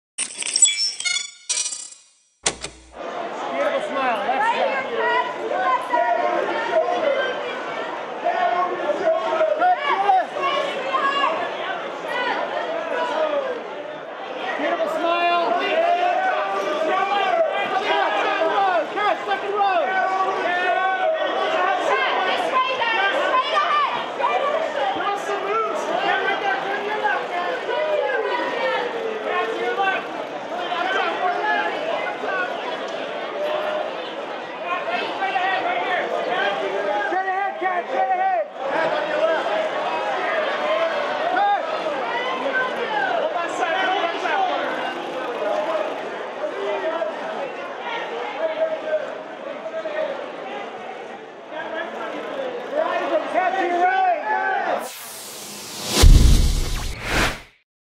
Kat Graham 2017 American Music Awards Red Carpet
Broll: Kat Graham walks the red carpet at the American Music Awards held at the Microsoft Theater in Los Angeles, California USA on November 19, 2017